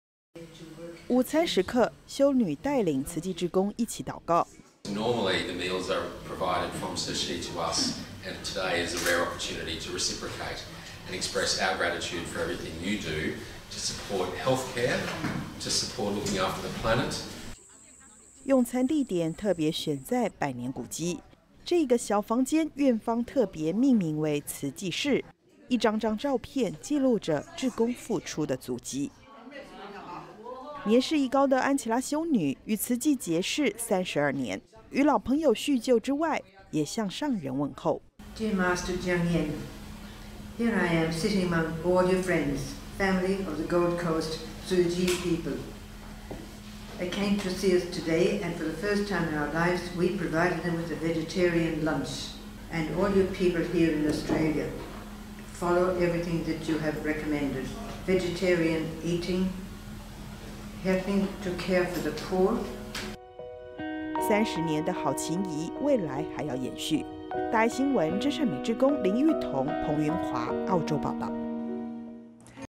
愛的關懷綿長久遠 澳洲醫護共餐感恩
澳洲慈濟志工與布里斯本瑪特醫院結緣32年，疫情期間，志工送上素食餐點給醫護人員打氣。志工這次到訪，院方特別準備西式素食餐點，感謝他們過去的幫忙。

午餐時刻，修女帶領慈濟志工，一起禱告。

瑪特醫院執行長 安德魯：「通常餐點都是由慈濟來提供，今天有機會讓我們回饋，感恩過去你們所做的，在醫療上的援助，以及對地球的貢獻。」

用餐地點特別選在百年古蹟，這一間小房間，院方特別命名為慈濟室，一張張照片，紀錄著志工付出的足跡。

年事已高的安琪拉修女，與慈濟結識32年，與老朋友敘舊之外，同時也向上人問候。

安琪拉修女：「親愛的證嚴上人，我在這裡，與黃金海岸的慈濟家人歡喜見面，第一次我為他們提供素食餐點，您所帶領的澳洲慈濟人，跟隨您的囑咐，實踐茹素飲食，幫助需要的人，救助貧苦。」

30載的好情誼，未來還要延續。

真善美志工 林昱彤 彭雲華 澳洲報導